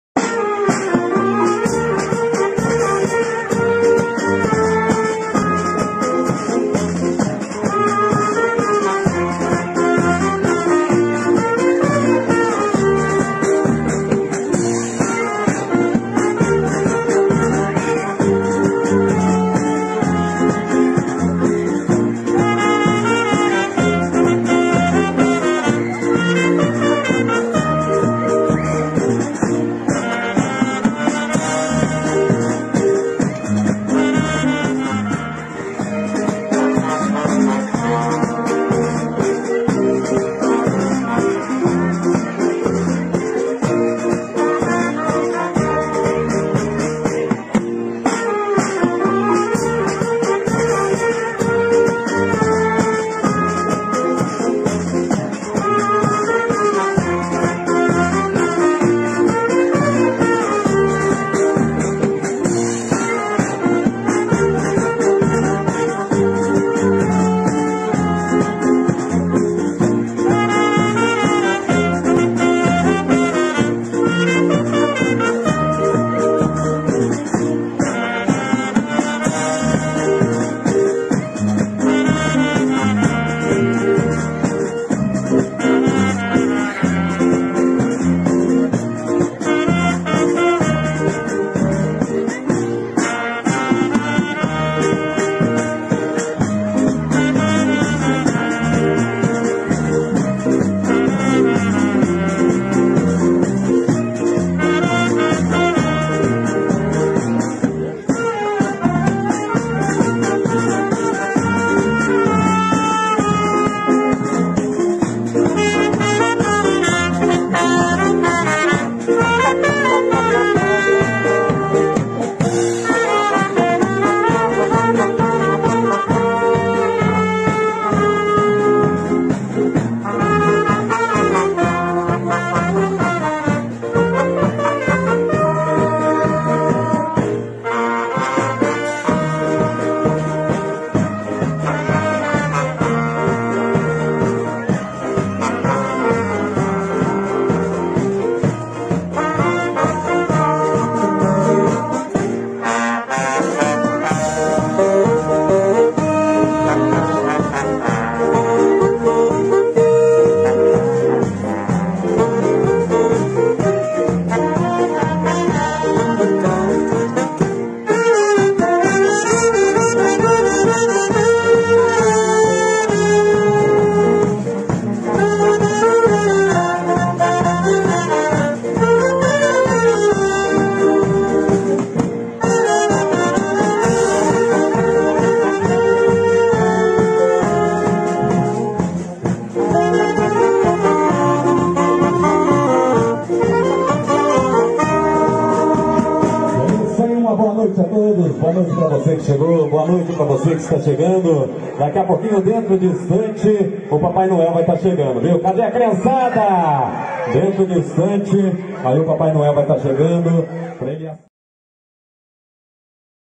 Boas Festas- Banda Musical Ouro Fino na cidade de Munhoz/Mg 😊🎶🎷🎅
Apresentação da Banda Musical Ouro Fino juntamente com integrantes da Lira Santa Cecília de Bueno Brandão neste sábado dia 11 na cidade de Munhoz/Mg na tradicional chegada do Papai Noel e acender das luzes 🎶🎺🎷🥁 Maestro Raul Barbosa.